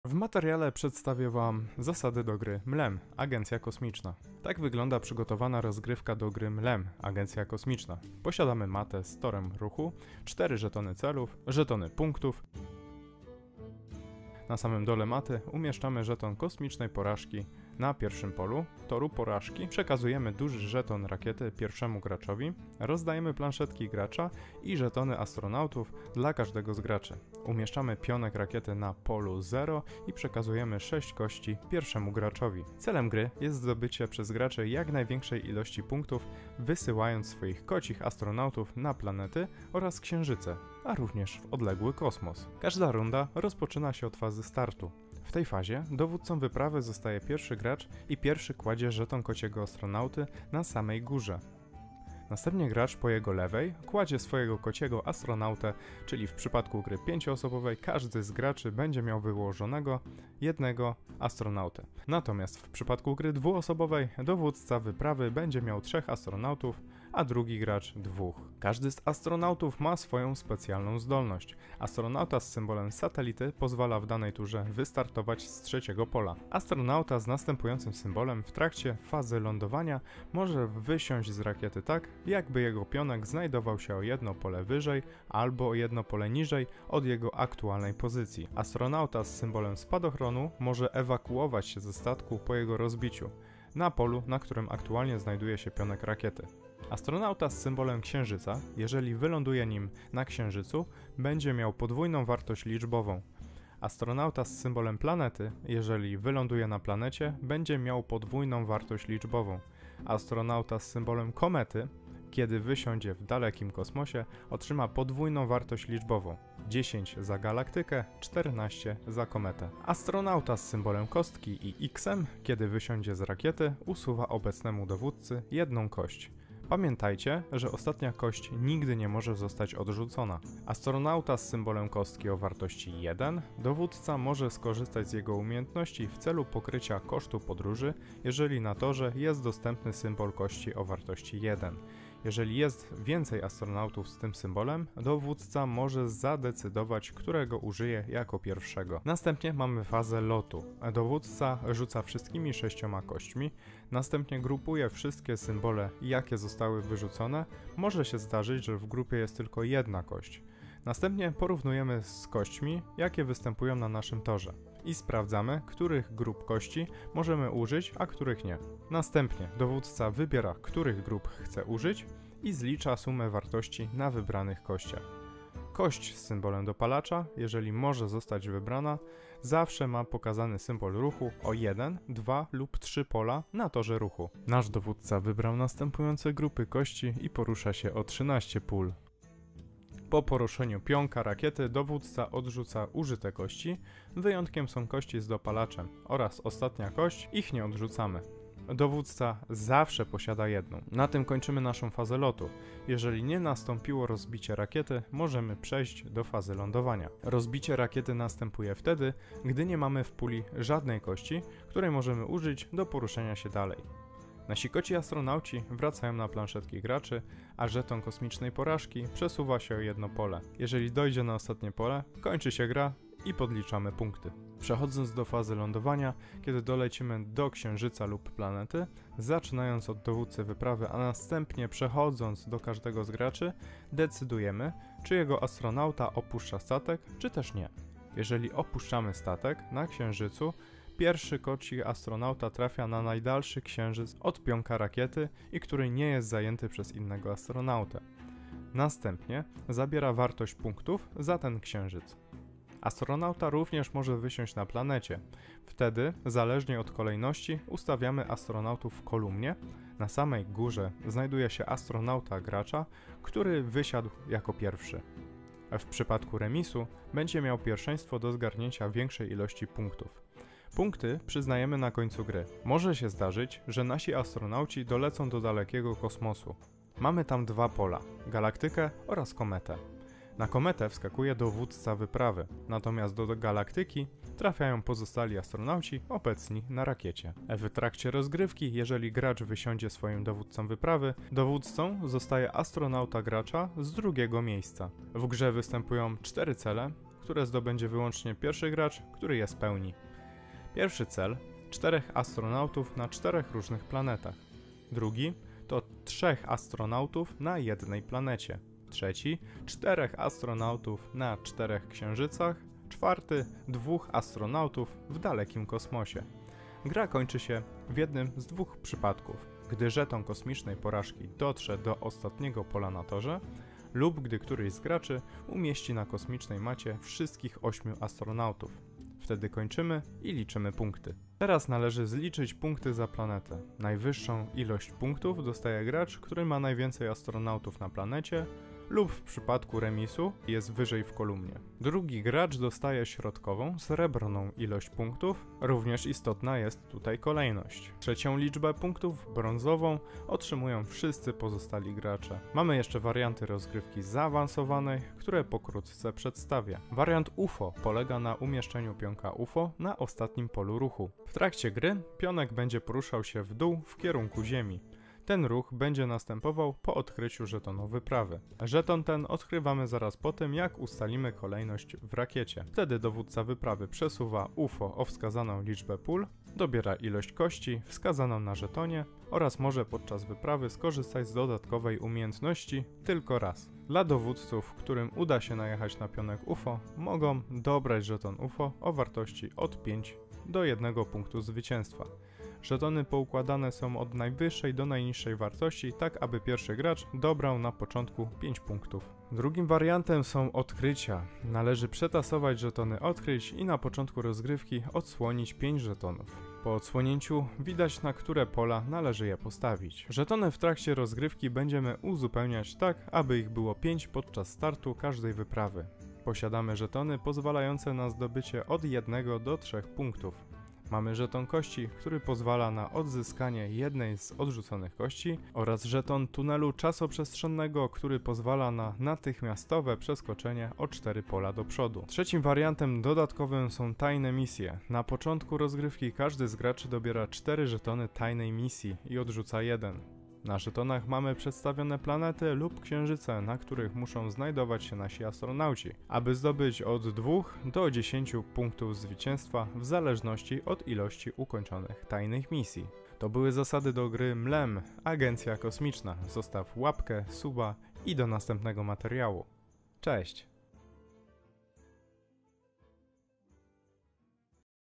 MLEM Agencja Kosmiczna | ZASADY GRY
Witajcie, miłośnicy kosmicznych przygód! Dziś przenosimy się w odległe rejony kosmosu, aby poznać zasady gry "MLEM: Space Agency"! Czy jesteście gotowi na ekscytującą podróż w towarzystwie nieustraszonych kosmicznych kotów?

W tej rodzinnej grze planszowej wcielicie się w role niezwykłych kapitanów kosmicznych wypraw, ale uwaga – są to koty o niezwykłym kocim charakterze! Od dawna podbijają one Ziemię swoją odwagą, a teraz ich celem jest opanowanie całego kosmosu. Jednak czy ich niezdarność nie wprowadzi chaosu w nasze kosmiczne eskapady?

W moim filmie przekazuję Wam wszystkie niezbędne informacje dotyczące zasad tej fascynującej gry. Poznajcie mechanikę rozgrywki, strategie, które mogą Was prowadzić do zwycięstwa, oraz niebezpieczeństwa czyhające na drodze do kosmicznej chwały.

Czy uda Wam się przejąć ster kosmicznej rakiety i sięgnąć gwiazd, czy też wasz koci instynkt doprowadzi Was na kocie manowce? Odpowiedzi na te pytania znajdziecie w moim filmie!

Po więcej materiału :)

0:00 - WSTĘP
0:15 - PRZYGOTOWANIE
0:45 - FAZA STARTU
2:28 - FAZA LOTU
3:44 - FAZA LĄDOWANIA
4:55 - CELE MISJI
5:55 - WARIANTY ZAAWANSOWANE